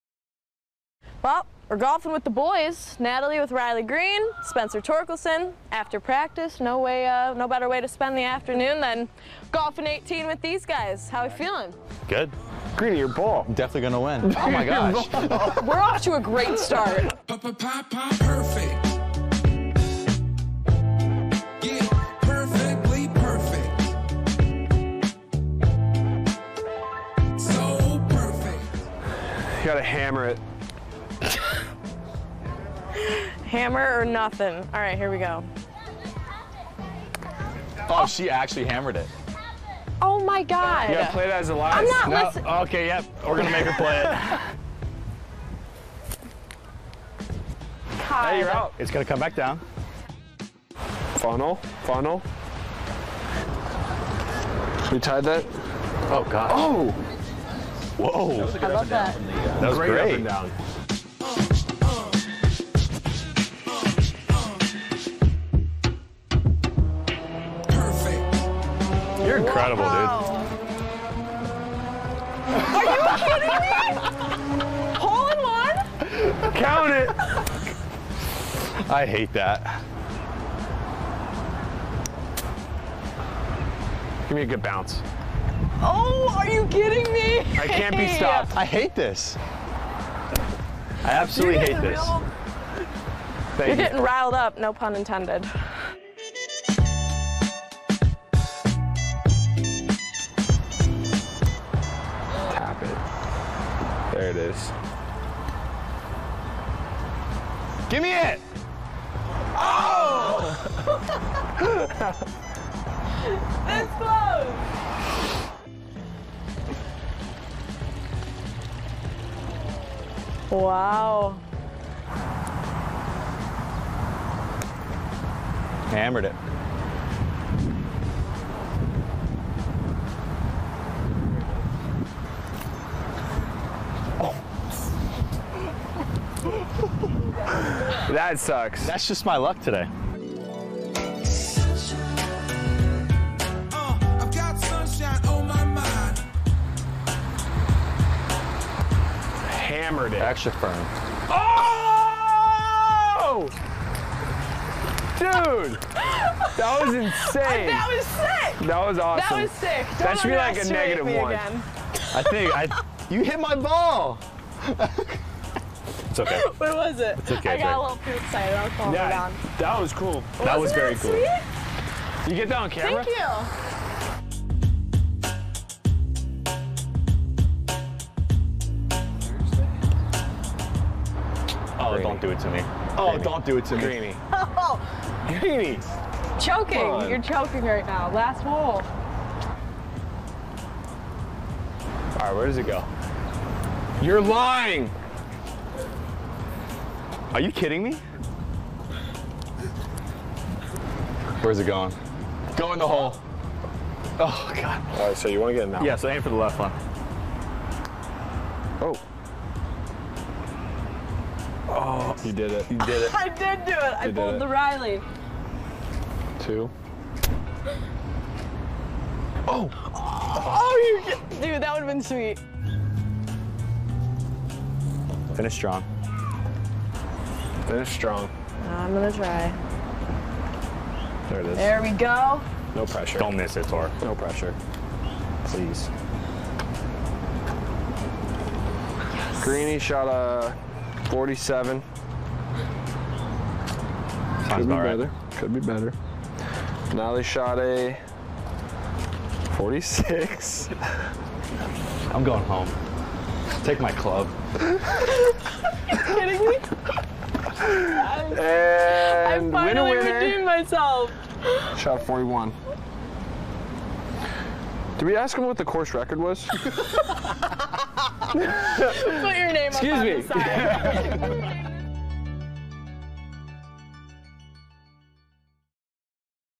Playing putt-putt
Before the Tigers broke camp, our Natalie Kerwin challenged Spencer Torkelson and Riley Greene to a round of putt-putt golf.